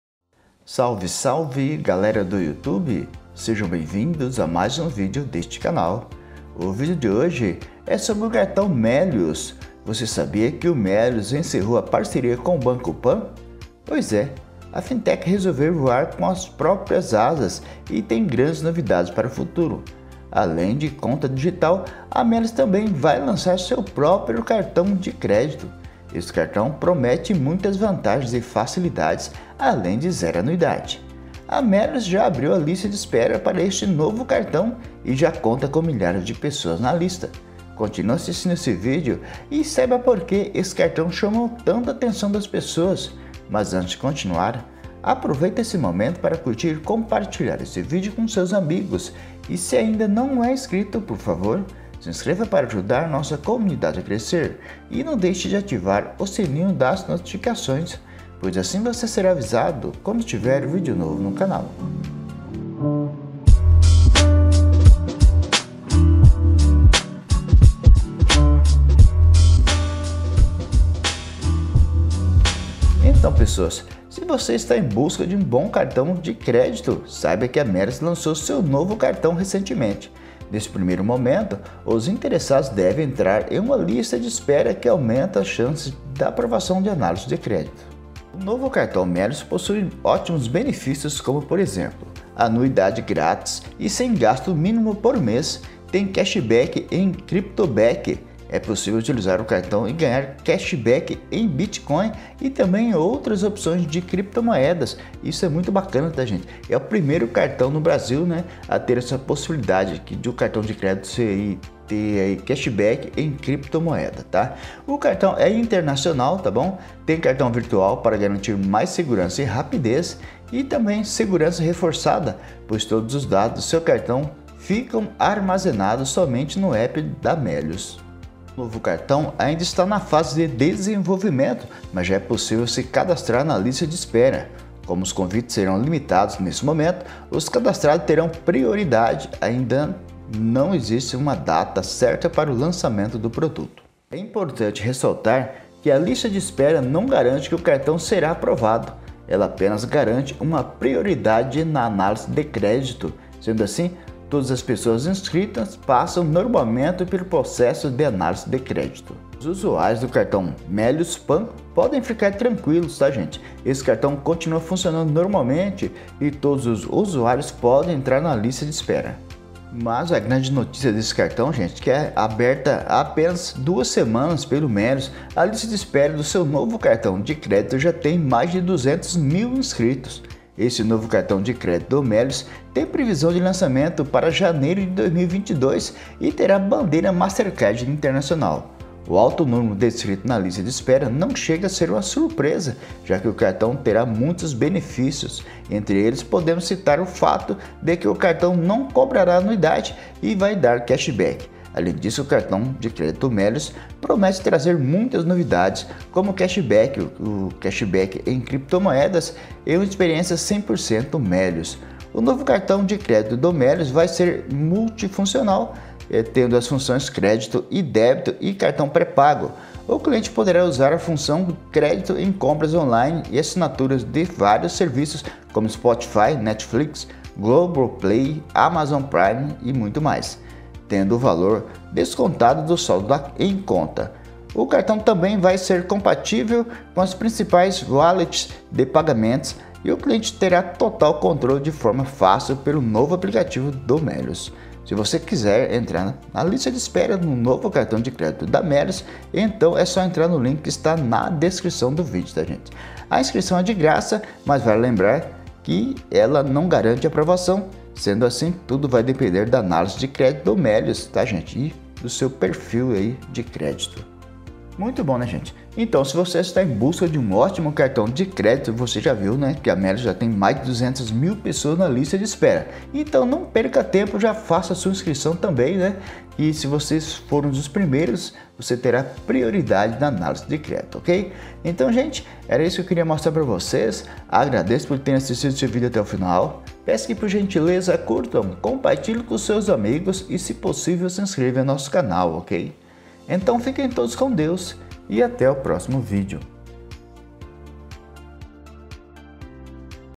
Está chegando cartão Meliuz zero anuidade, saiba como ter o seu
Cartão internacional zero anuidade pra sempre
Se você está em busca de um novo cartão de crédito, saiba que a Méliuz lançou seu novo cartão recentemente. Neste primeiro momento, os interessados devem entrar em uma lista de espera que aumenta as chances de aprovação na análise de crédito. Também conseguem entrar na lista de espera quem possui o cartão Méliuz PAN.